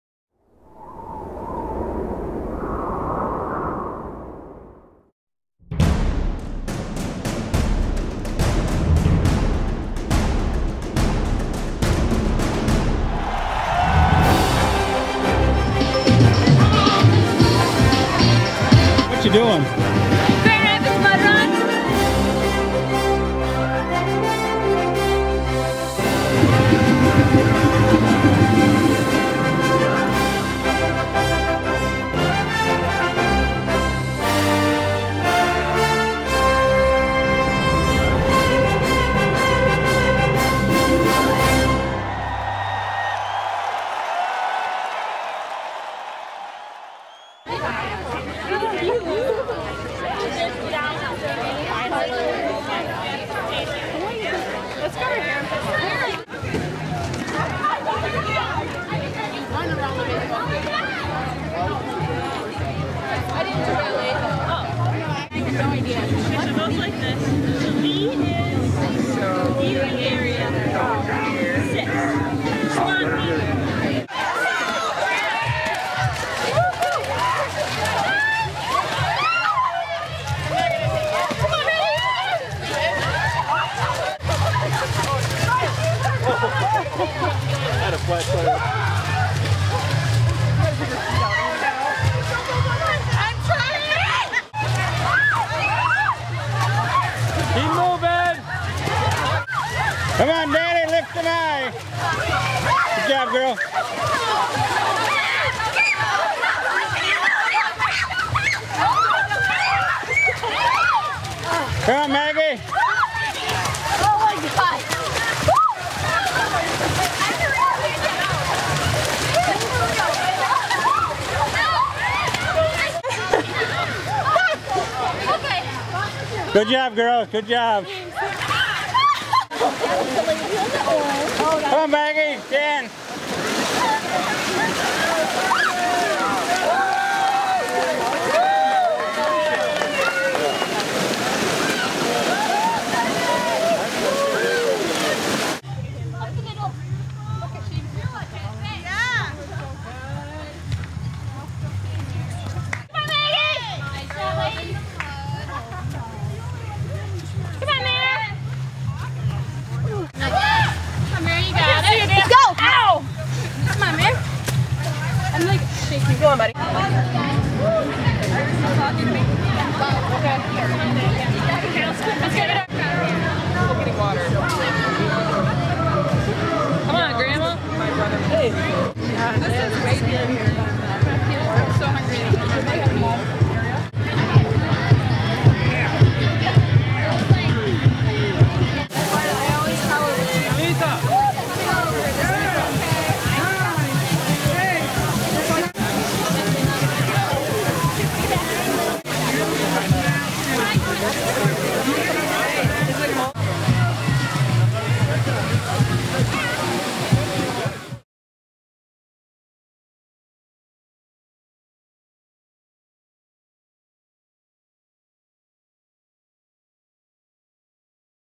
Grand Rapids Mud Run 2011
The Grand Rapids 5k Mud Run on 27 August 2011